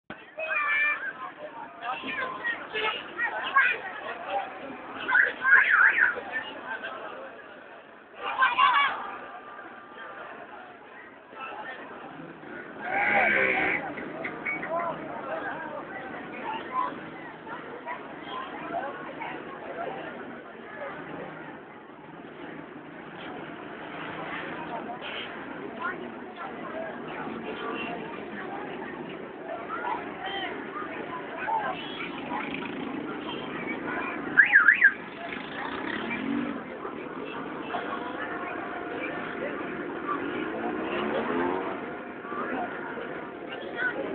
Sohawa (Jhelum) Pakistan Lights on Eid Milad-un-Nabi
Lights in Sohawa Town (Jhelum) on Eid Milad-un-Nabi